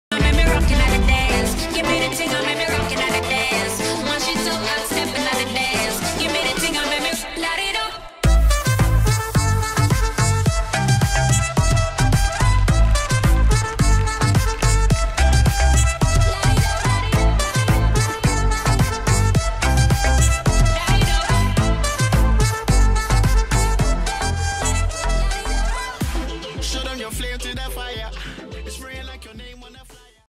Car Towing Skiers and Snowboarders in Boone, NC
We took advantage of all the snow and decided to try and tow some friends with the car. Turned out pretty well and we had a a lot of fun.